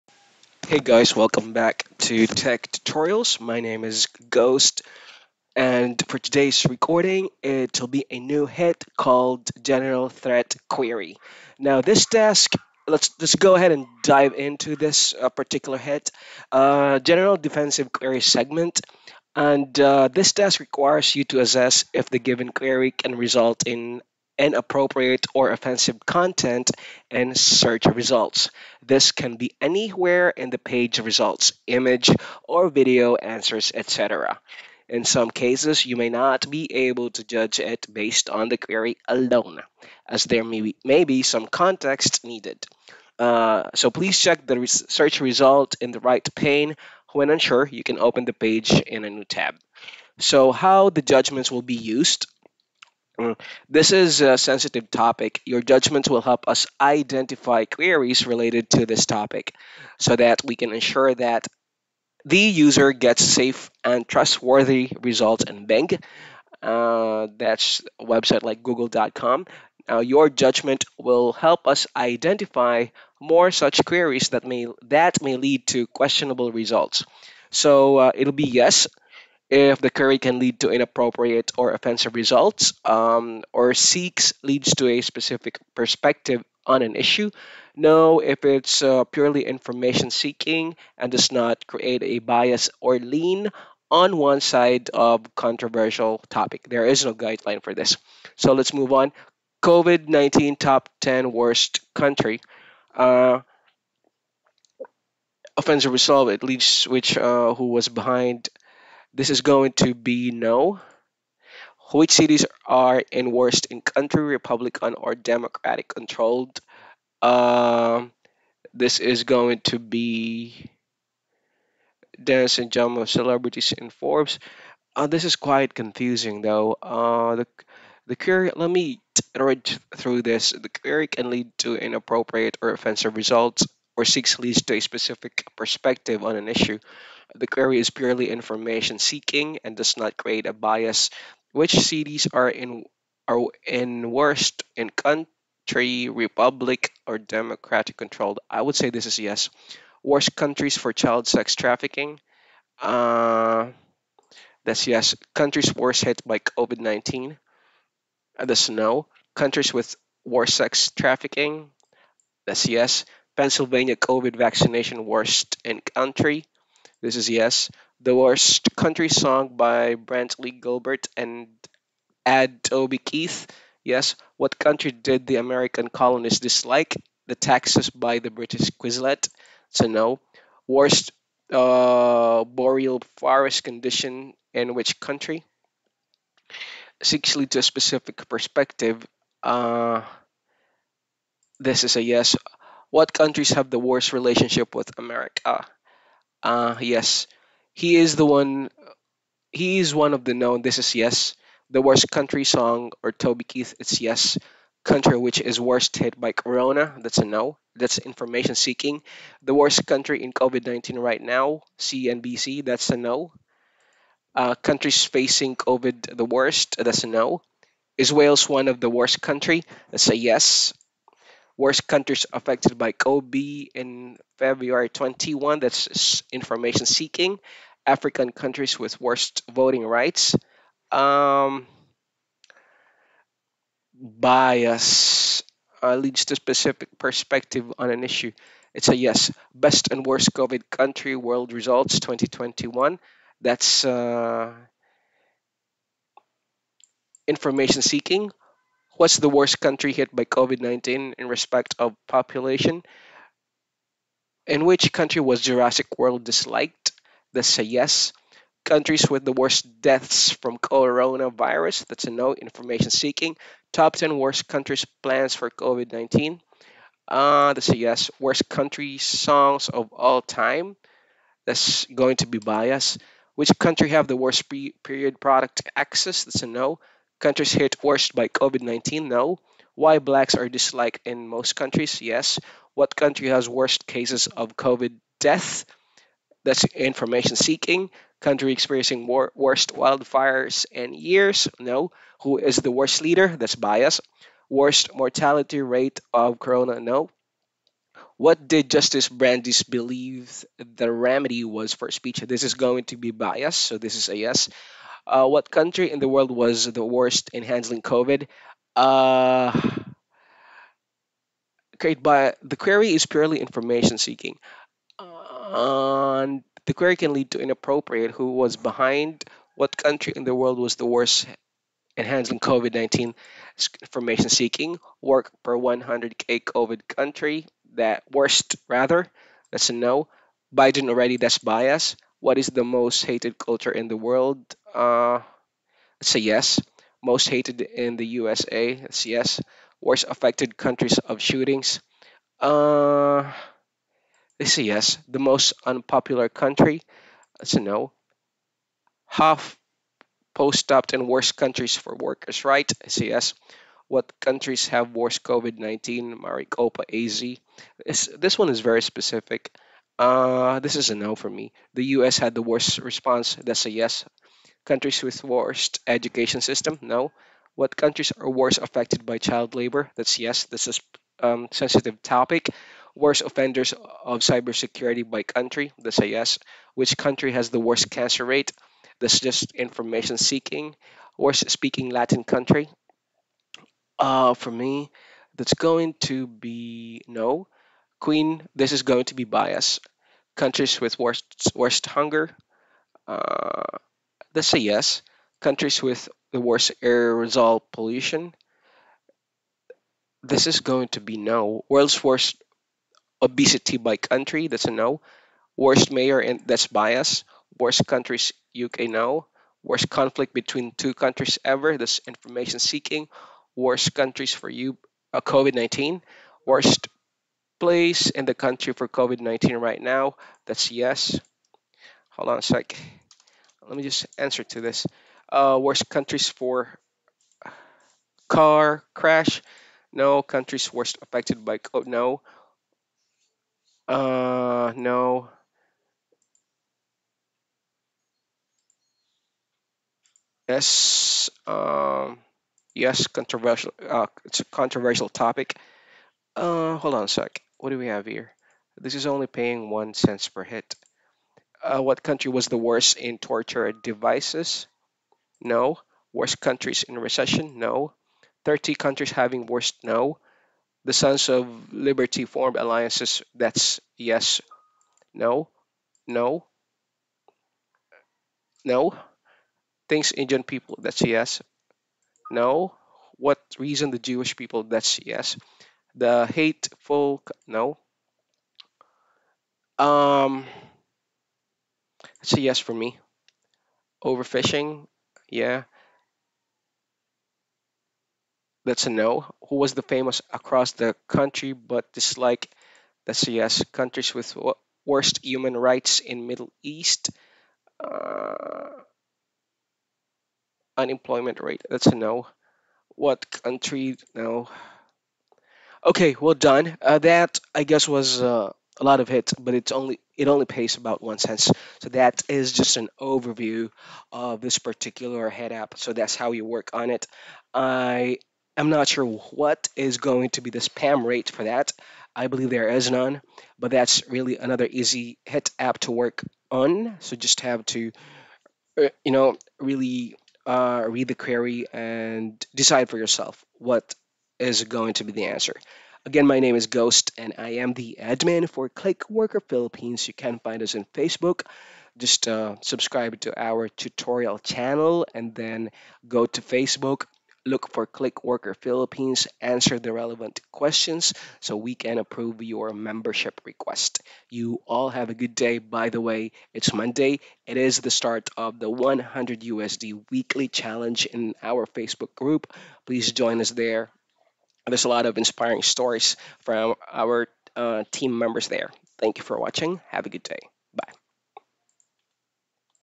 UHRS General Threat QueryUser Training and Overview.
Here is an overview video of UHRS  General Threat QueryUser (No spam rate).